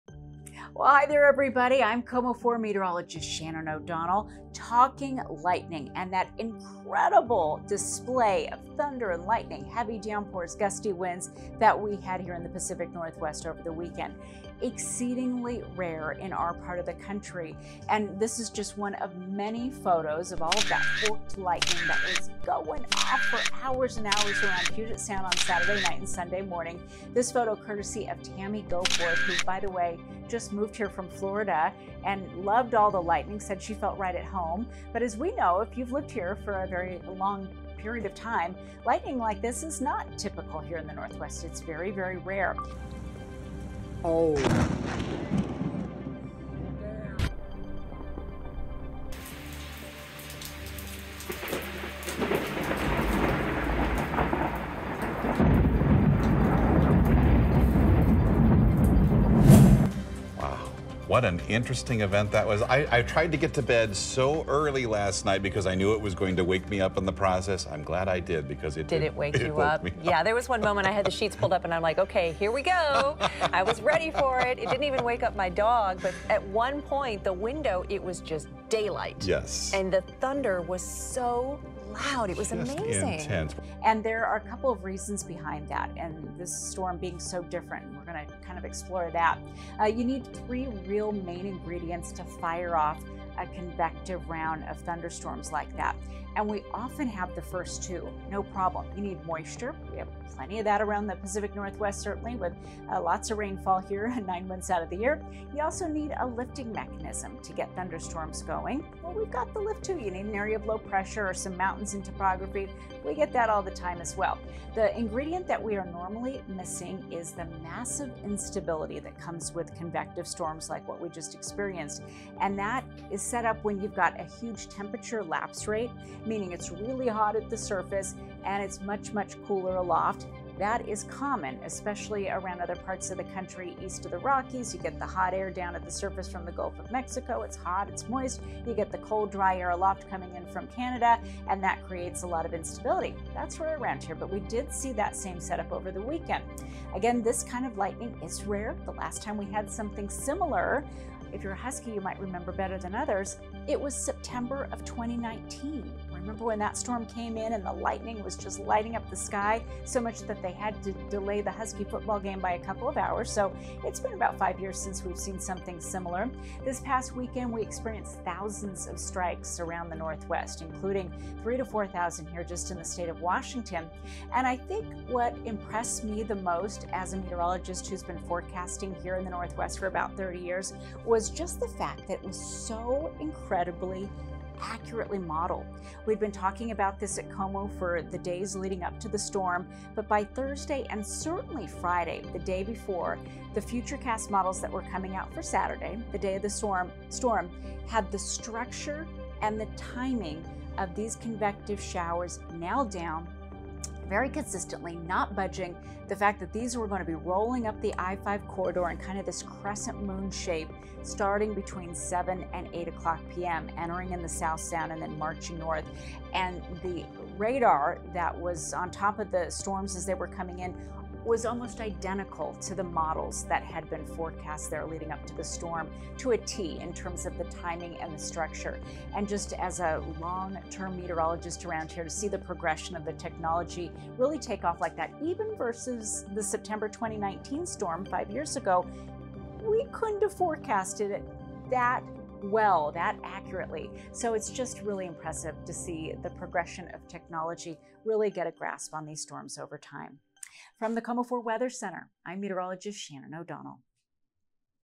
QUITE THE SHOCK: Here's the science behind Washington's rare lightning storms
The most energized and most exciting part of the thunderstorms on Saturday is over, but many people around Puget Sound captured moments when lightning struck this past weekend. KOMO Chief Meteorologist Shannon O'Donnell takes a look at how and why this nature phenomena is so rare in the Pacific Northwest.